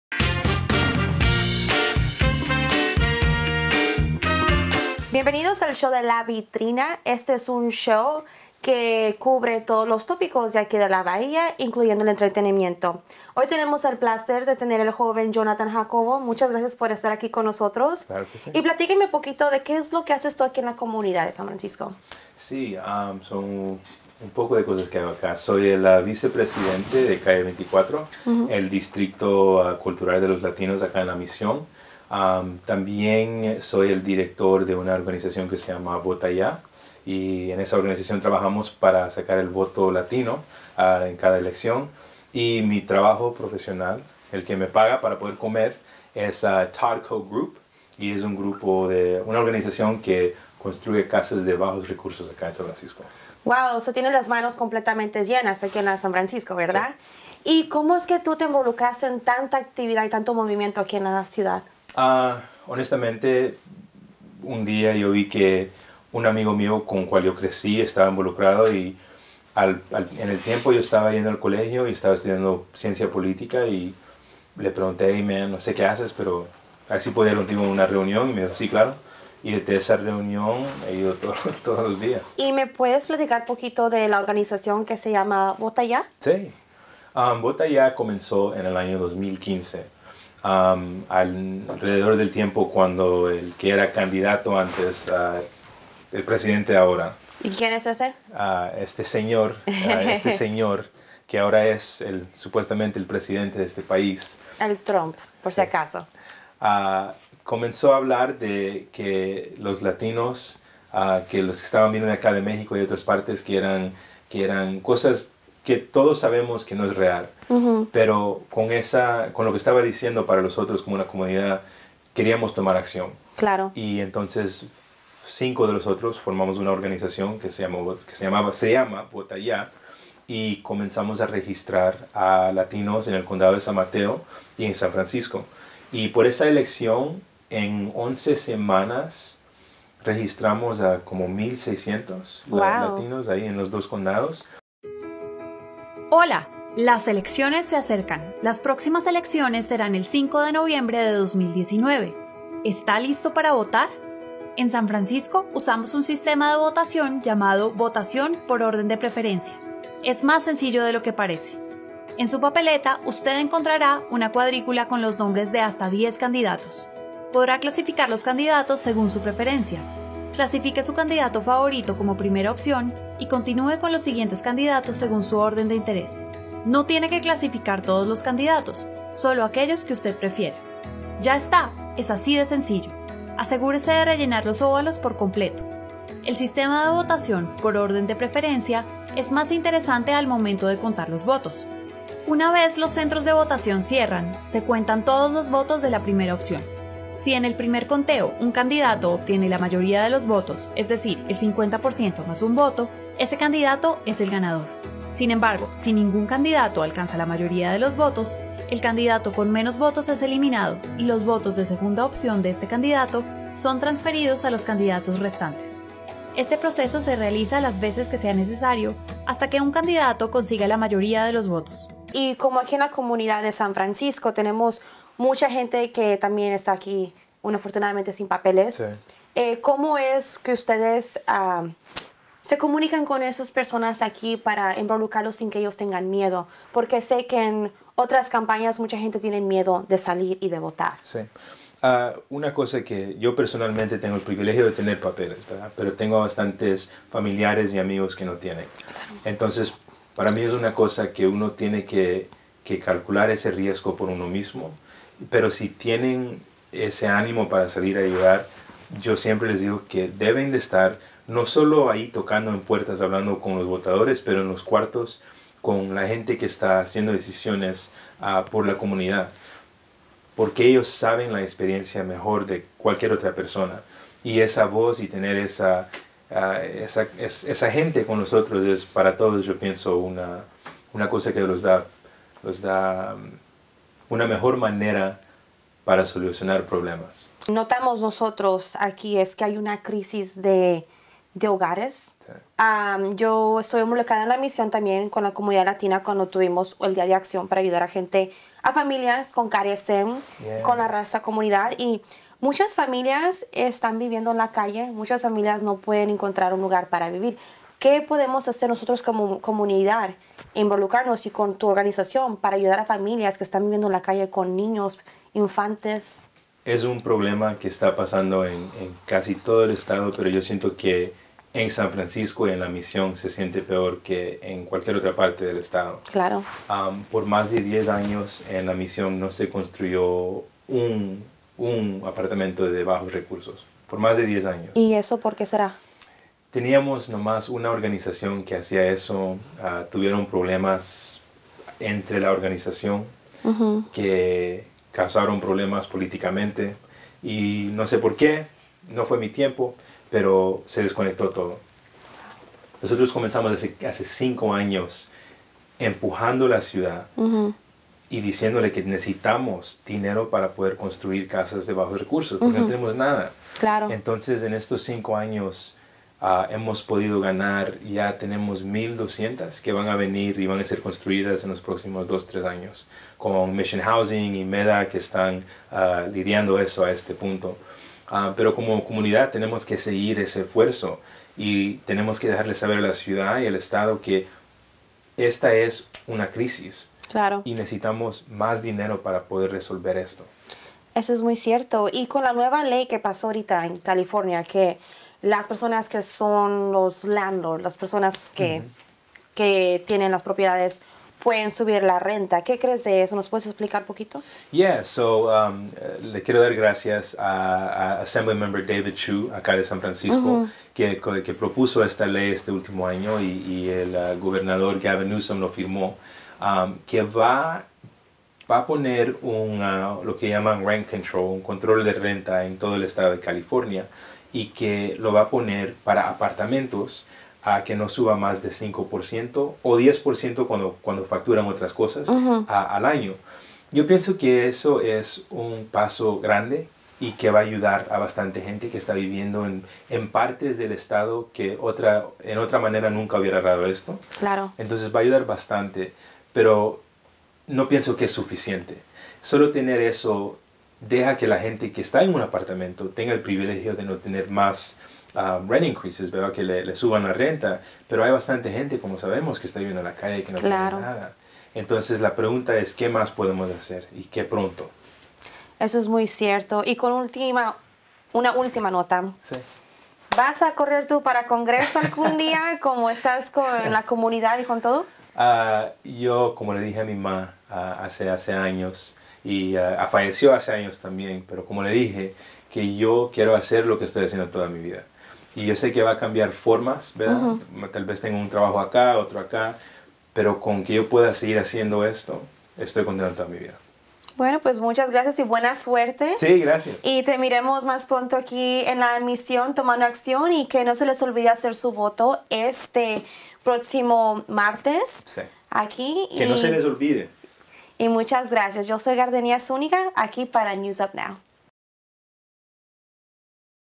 Jon Jacobo: un joven que está teniendo un gran impacto en la comunidad
Tuvimos el privilegio de tener, en nuestro estudio, Jon Jacobo que está teniendo un gran impacto en la comunidad. Se lo invocó con Fundador y Director Ejecutivo, Vota Ya. Hablamos sobre temas importantes como la vivienda economica, la importancia de votar, el movimiento político Latino y estrategias sobre cómo organizarse.